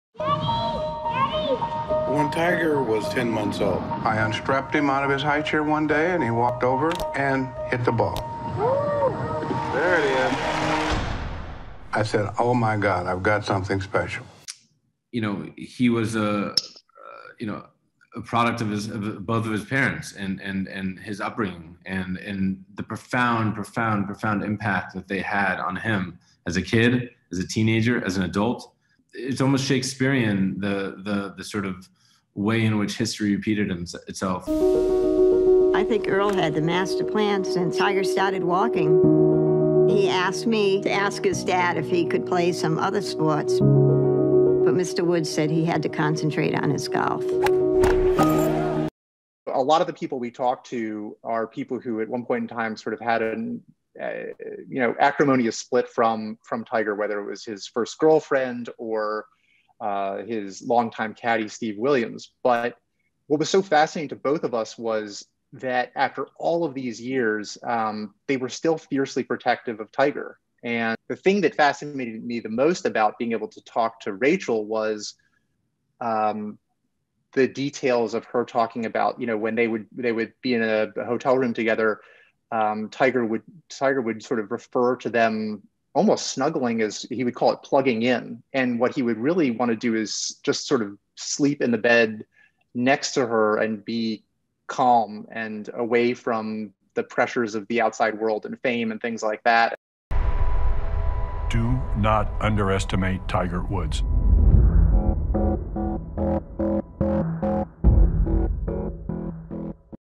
Tiger Woods Uncovered - New documentary to be released
A new documentary on the rise and fall of Tiger Woods reveals details about the pro-golfer from people who knew him intimately.

"Tiger" is a two-part series on HBO that looks at Woods' love and talent for golf from the age of two, his intense relationship with his father, Earl Woods, and the many championships he won after working relentlessly on his game. But the series also examines who he was when out of the spotlight: a playful teenager who yearned for a life beyond golf, a fierce competitor who revered his father but also shut him out in later years, and a vulnerable man who needed an escape from the media and the pressure to win.

Directors Matthew Heineman and Matthew Hamachek used Jeff Benedict and Armen Keteyian's best-selling book "Tiger Woods" as a base for their story, but worked hard to get many who knew Tiger well to speak on camera.

"A lot of the people we talked to are people who at one point in time sort of had an acrimonious split from from Tiger, whether it was his first girlfriend or his long time caddy, Steve Williams. But what was so fascinating to both of us was that after all of these years, they were still fiercely protective of Tiger. And they've seen, I think, a lot of people in the media, you know, sort of tear him down," Hamachek told SNTV in a recent interview.

"When you have a front row seat to Tiger's life, like a lot of these people did, you see that from the age of two onward-- whether it was Earl, his father, or Nike in their advertisements, or the sort of public at large-- people were constantly projecting things onto him."

The series includes never-before-seen footage and interviews with those who know Tiger best, including his former caddy and close friend, Steve Williams, Tiger's first true love, Dina Parr; as well as Rachel Uchitel, the woman at the center of the sex scandal that eventually brought Tiger down and led to the end of his marriage. Uchitel speaks on camera for the first time in years and describes the time she spent with Tiger, often in hotel rooms where he could escape his life for a while.

"What's sort of fascinating about Tiger is that he is probably one of the most recognizable faces on the planet, but I don't think the public really knew much about him at all beyond what what was presented to us by Nike. And so, you know, the process of making a documentary is really listening to the people that were, that were there in person and really knew him well," Hamachek said.

Tiger will premiere on HBO January 10th.